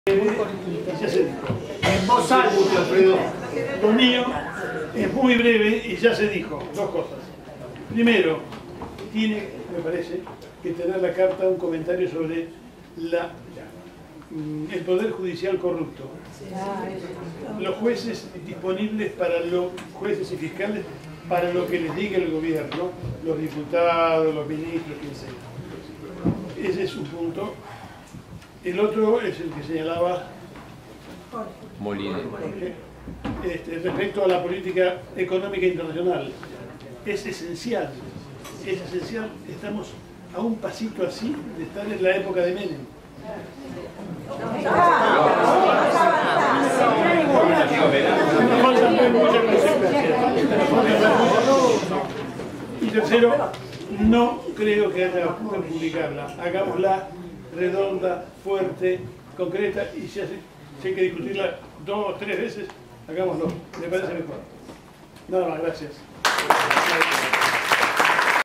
15CA Alfredo Vidal
Discusión del borrador de la Carta Abierta /21 Dar testimonio. Asamblea del sábado 25 en la Facultad de Ciencias Sociales de la UBA. 250616 CABA